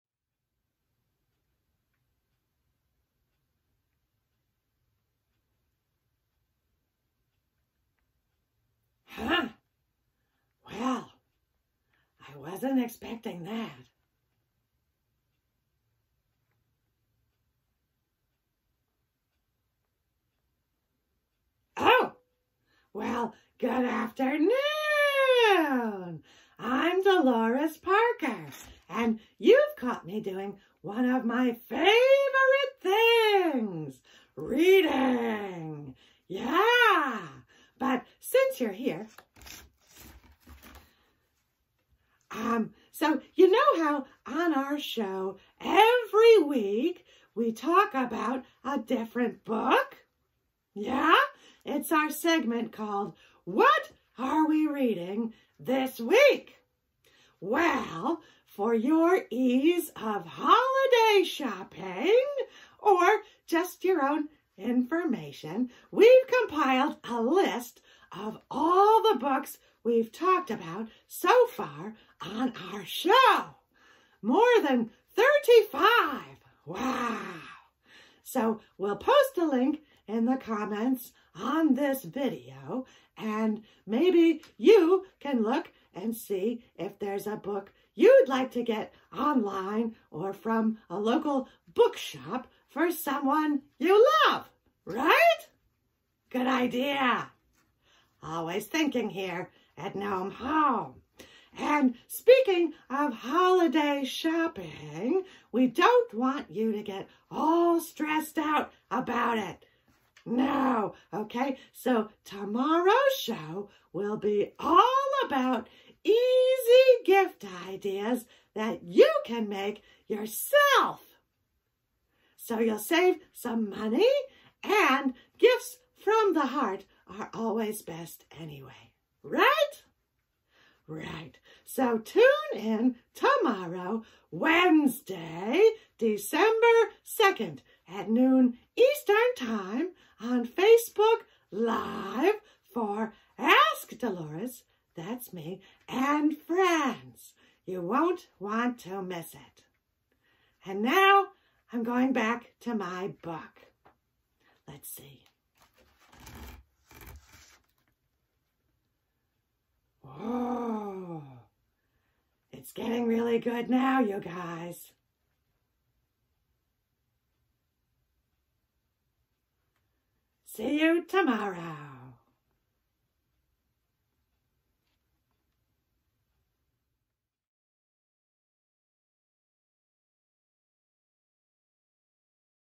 Dolores reads a book
Dolores Parker takes a break from reading to remind you to watch her Facebook Live show on Wednesdays at noon, and to check out her list of book recommendations!